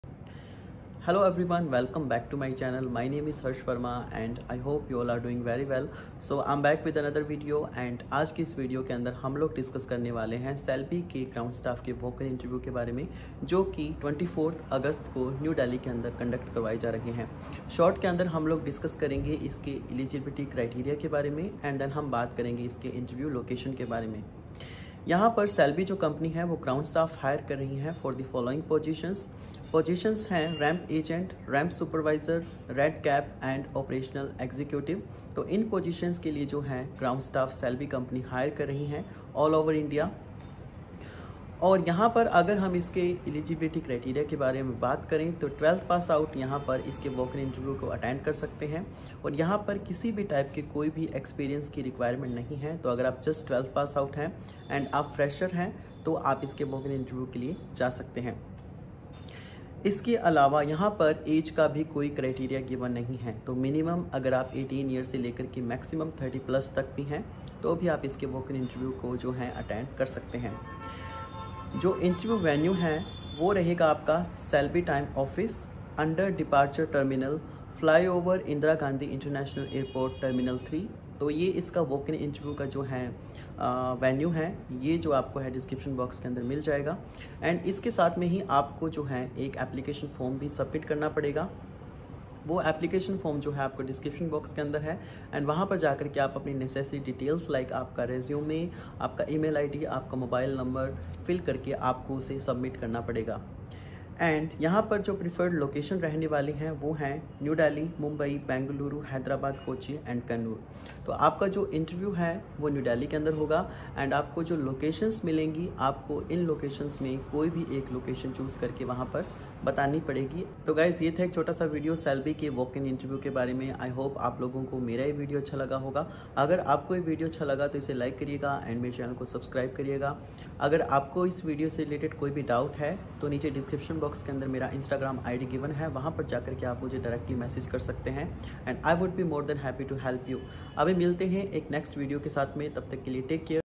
Celebi hiring ground staff freshers 2021 walk in interview ( HSC )
Interview date & venue 👉🏻

24th August 2021
CELEBI time office ,Under  ￼departure terminal , flyover IGI airport T-3 New Delhi